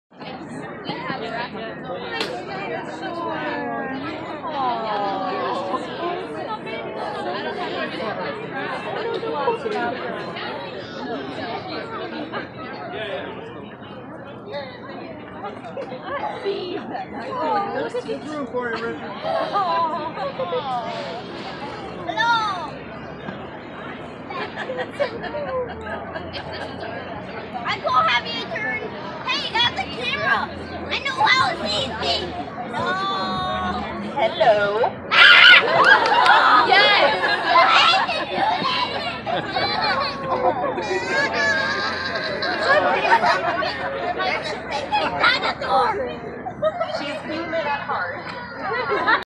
Raptor in snohomish Farmers Market 😍
The little ones are my favorite to make happy :)